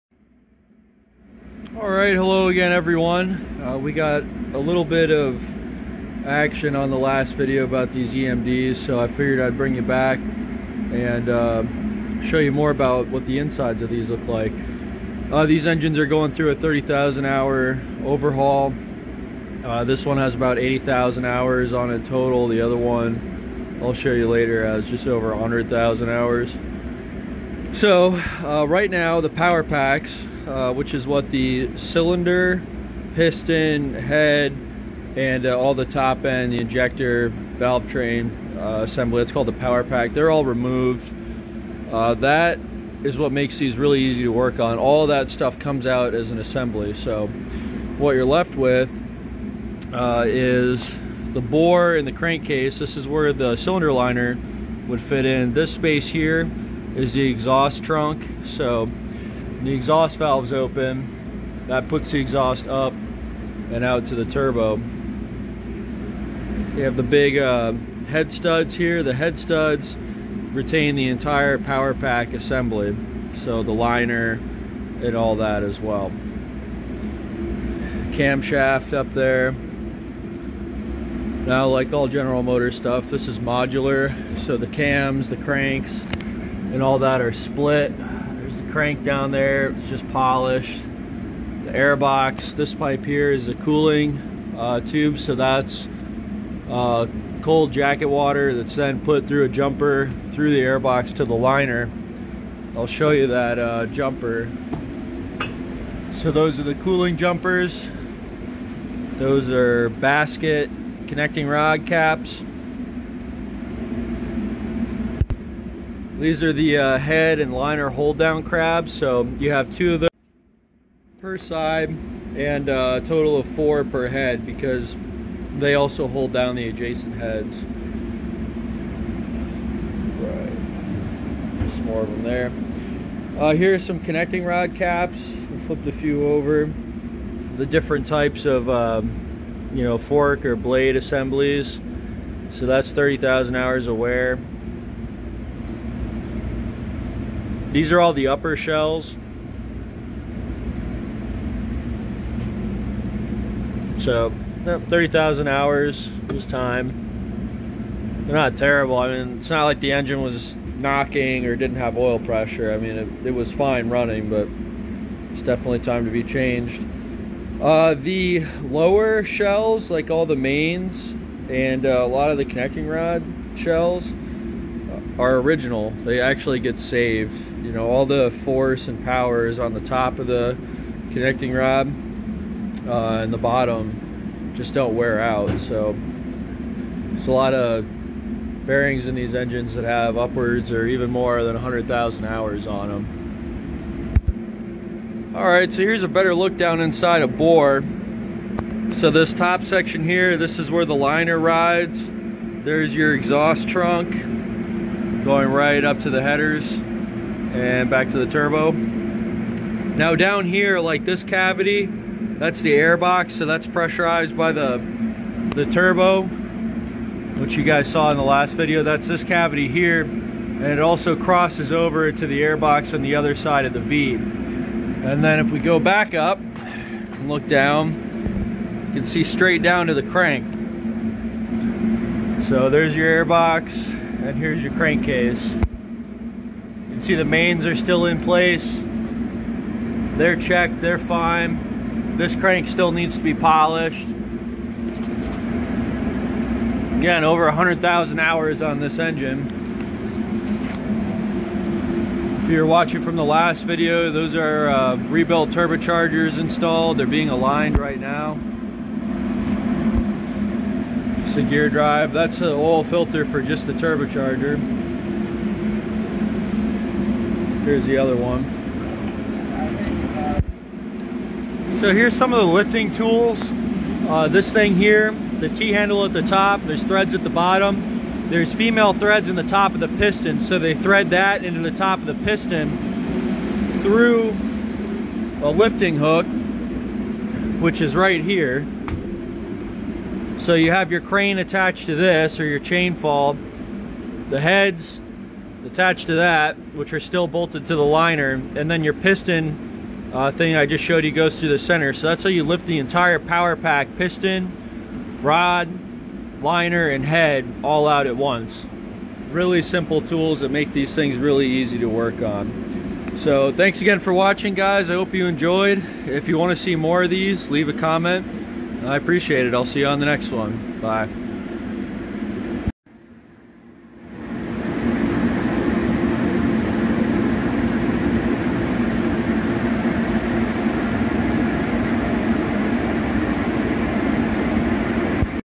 Rebuild of 2 Stroke EMD Ship Engines
30k Hour overhaul of 12 and 16 cylinder 645F7-B Electromotive Diesel marine generators. The 12cyl has over 100k hours of operation.